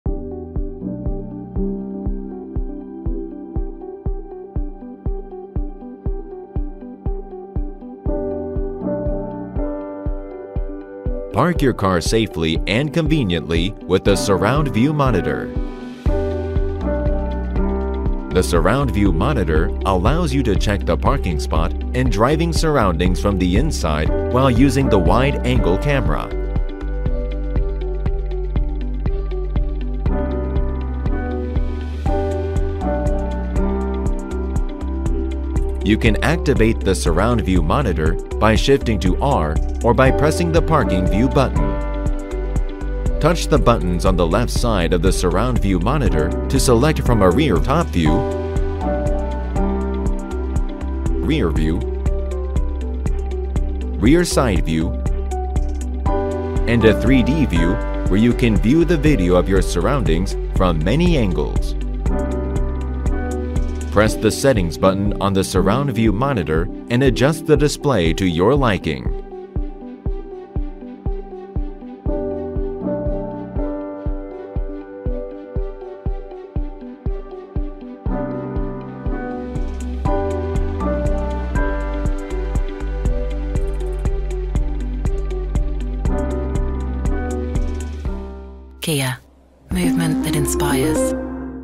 How to park with the Surround View Monitor | Kia How-To
Our Surround View Monitor is the easiest way for you to park safely. Anywhere. And Everywhere.
Lear how easy it is to use in this video.

Learn how easy it is to use in this video.